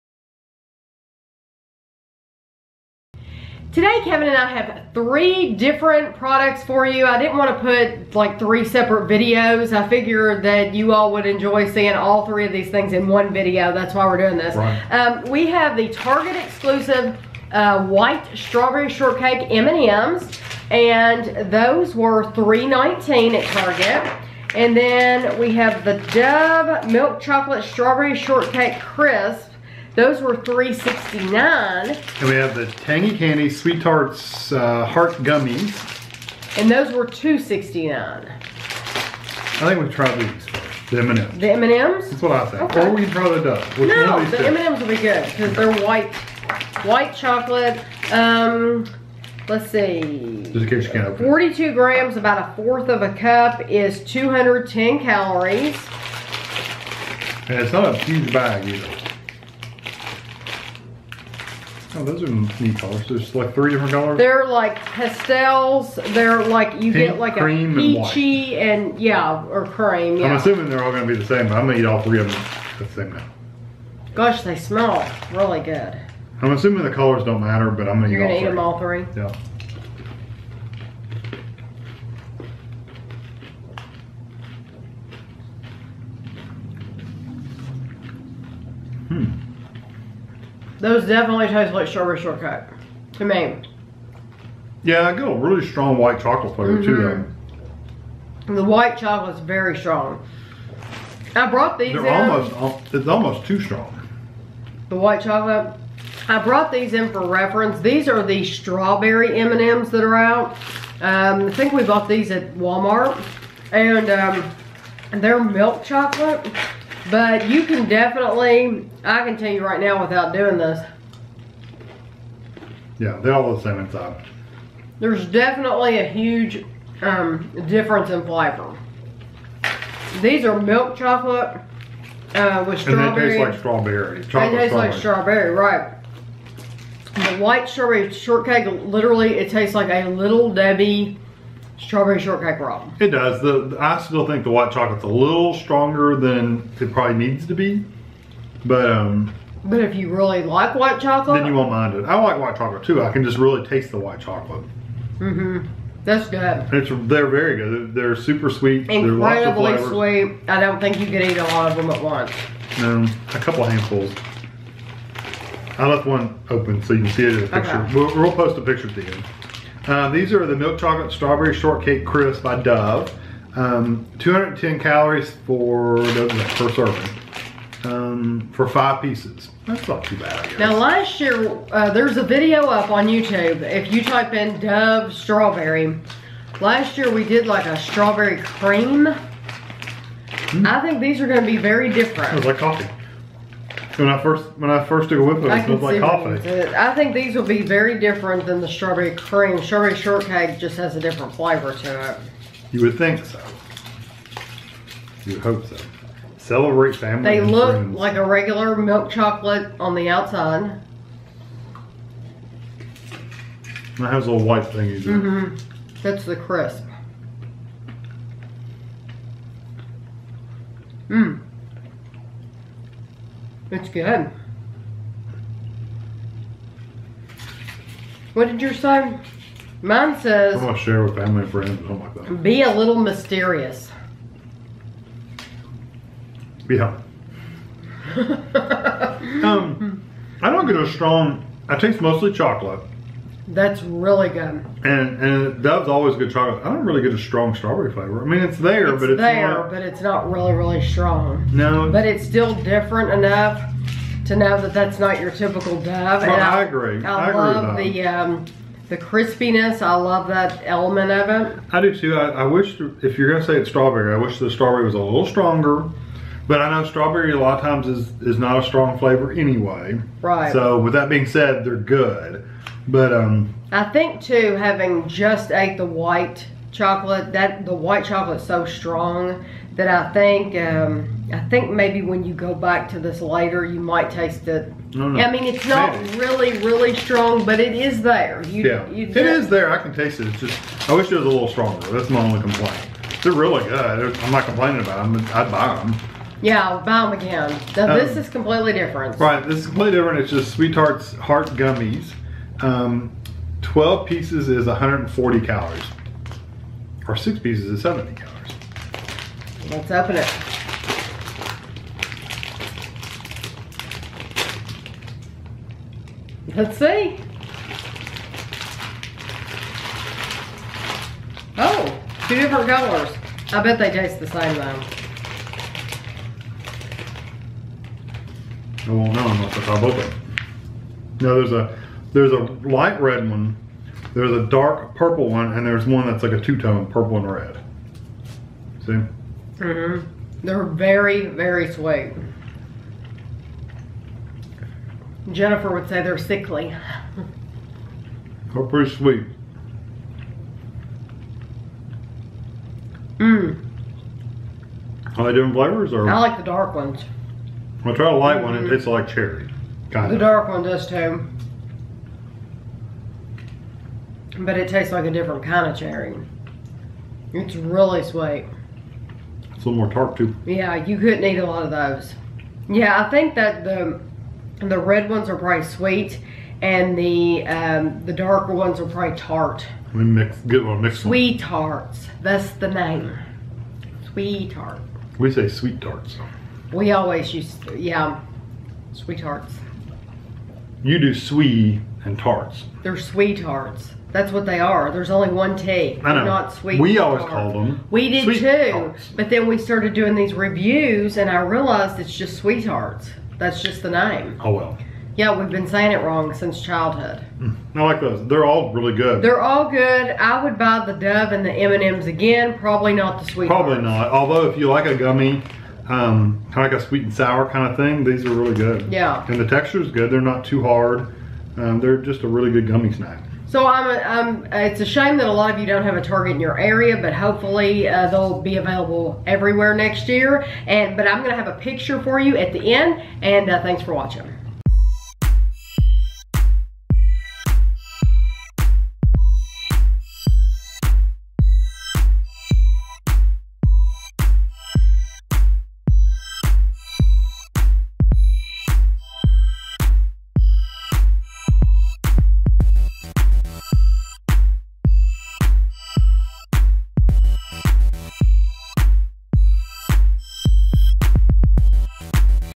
White Strawberry Shortcake M&M’s, Dove Strawberry Shortcake Crisp & SweeTarts Heart Gummies
This is a taste test/review of the Target Exclusive White Strawberry Shortcake M&M’s, the Dove Milk Chocolate Strawberry Shortcake Crisp bars and the SweeTarts Heart Gummies.
The White Strawberry Shortcake M&M’s were $3.19 at Target.
The Dove Strawberry Shortcake Crisp bars were $3.69 at Target.
The SweeTarts Heart Gummies were $2.69 at Target.